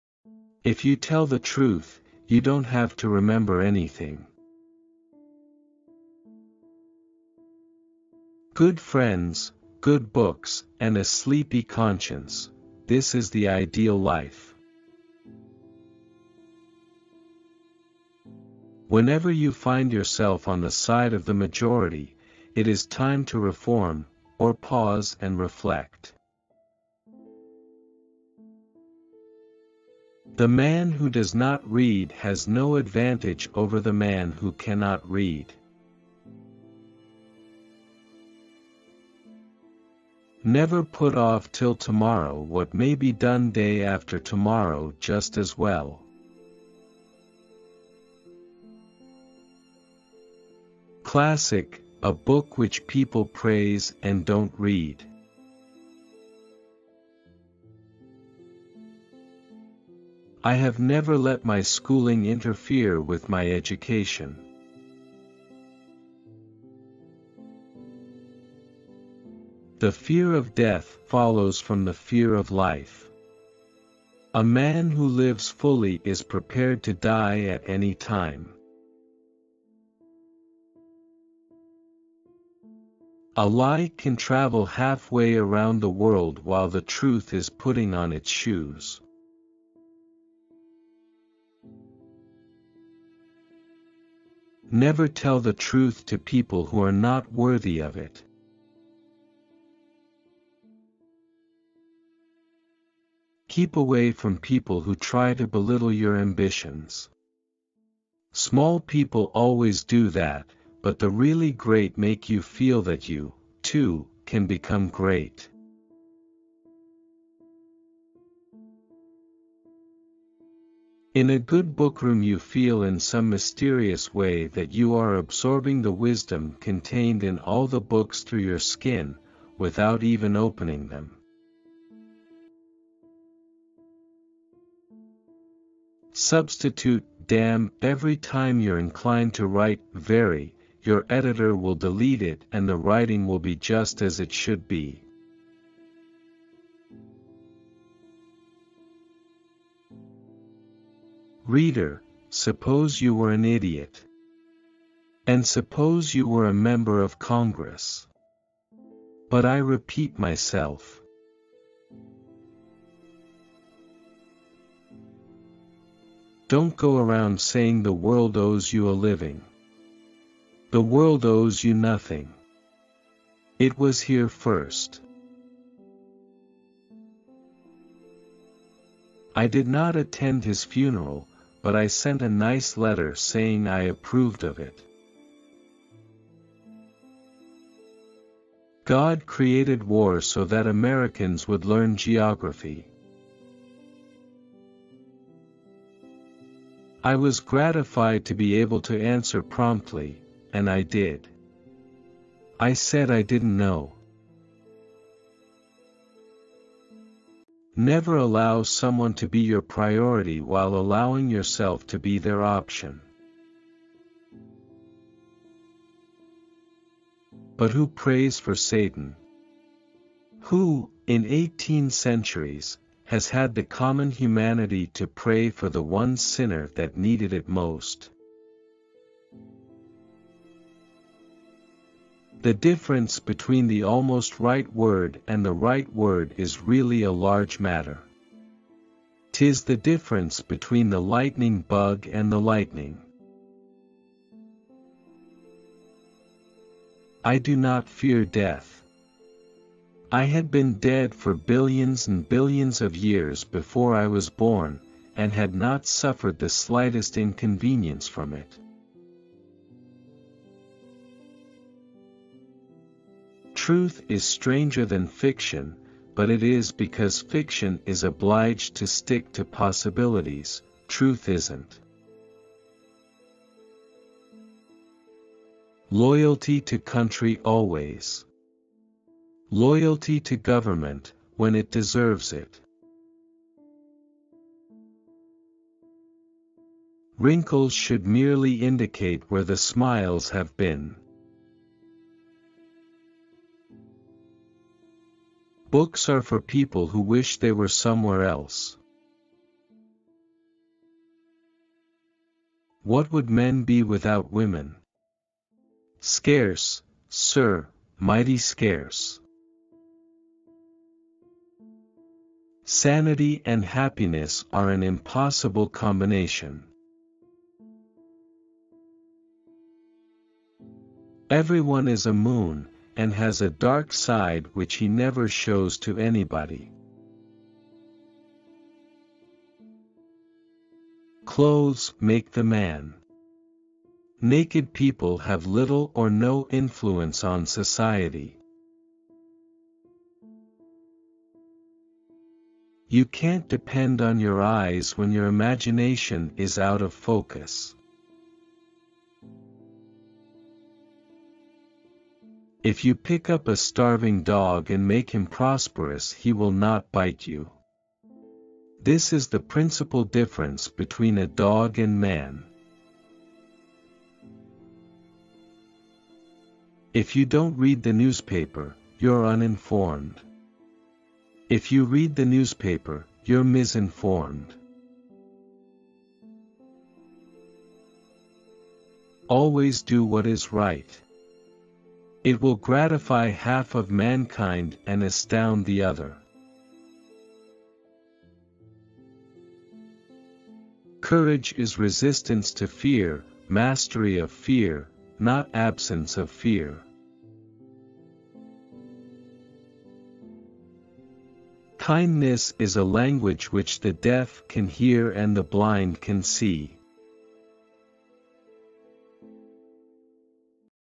36 Quotes from MARK TWIAN that are Worth Listening for You! | Life Changing Quotes of Mark Twain
36 Quotes from MARK TWIAN that are Worth Listening for You! | Life Changing Quotes of Mark Twain

Samuel Langhorne Clemens, known by his pen name Mark Twain, was an American writer, humorist, entrepreneur, publisher, and lecturer. He was praised as the "greatest humorist the United States has produced", and William Faulkner called him "the father of American literature".
Born: 30 November 1835, Florida, Missouri, United States
Died: 21 April 1910, Stormfield, Redding, Connecticut, United States
Full name: Samuel Langhorne Clemens
Children: Clara Clemens, Susy Clemens, Jean Clemens, Langdon Clemens
Buried: 24 April 1910, Woodlawn Cemetery, Elmira, New York, United States
Spouse: Olivia Langdon Clemens (m. 1870–1904)

 We hope you enjoyed this Wise Life Quotes of MARK TWIAN , a lot of work has been put into it.
Quotes for Motivate
happy new year
status video